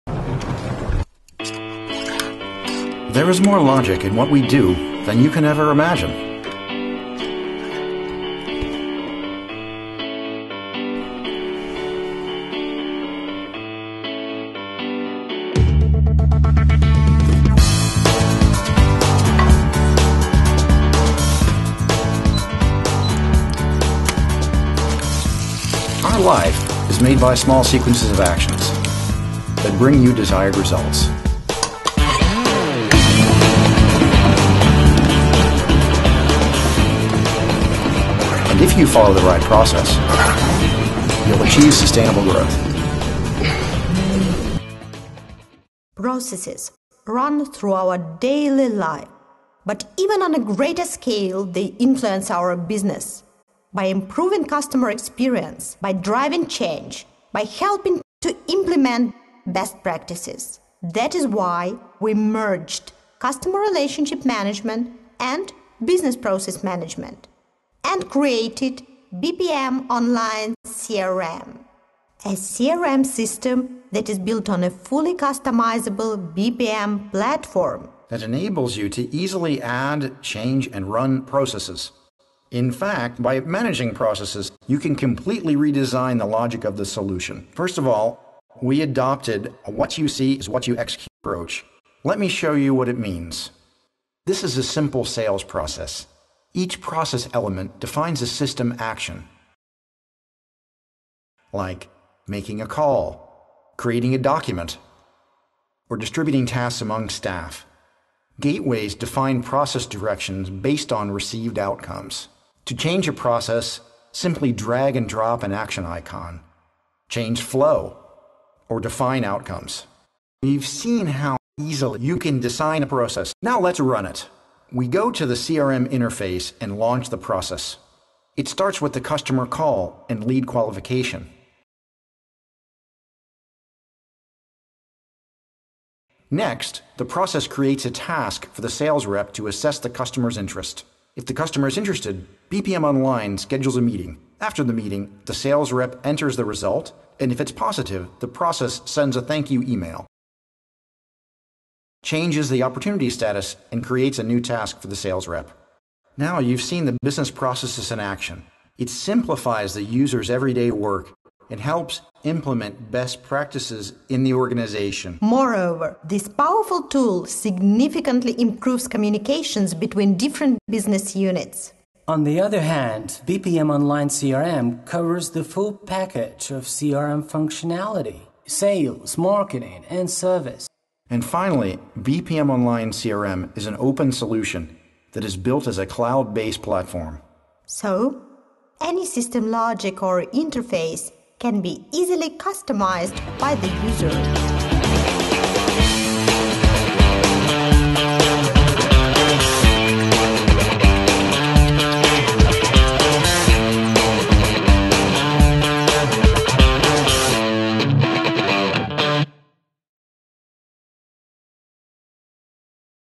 BPMOnline CRM presentation for CRM Idol
Task: Make presentation for BPMOnline software for CRM IDOL 2011 competition.
.. and they won it :)